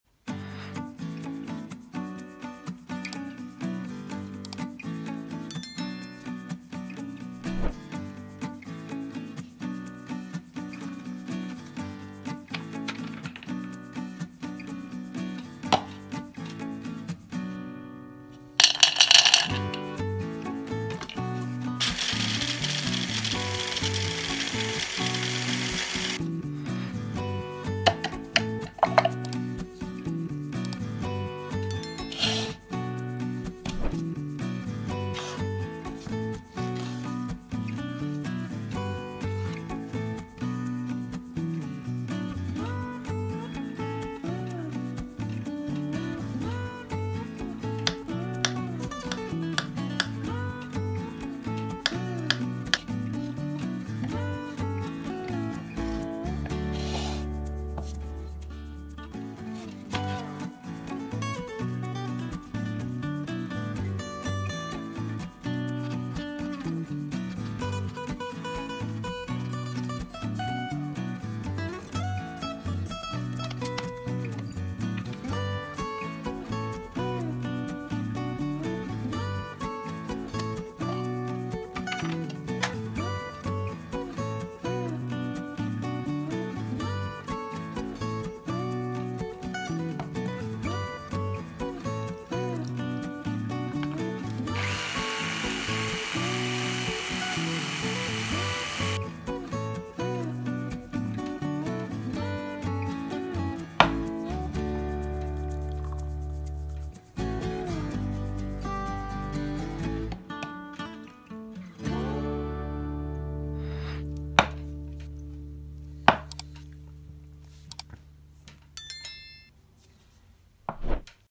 One glass and a boost of vitamins for the whole day
Ingredients:
1. Banana
2. Nuts
3. Ginger
4. Honey
5. Yogurt
✅ To learn the secrets of cooking, turn on the subtitles ⚙️ in your language.
Much luck!